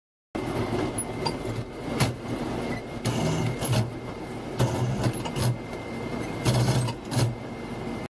2002 JOHN DEERE 7210 For Sale
This is a good starting, running and driving tractor but it has some issues.  The range transmission lever grinds in C and D range, but it does dirve in those gears.  It does not drive in E range.  The 3 point hitch will not go down.  Every thing else seems to work like it should during a quick drive around our yard..  Please look at all the pictures and watch all the videos.  Selling as-is.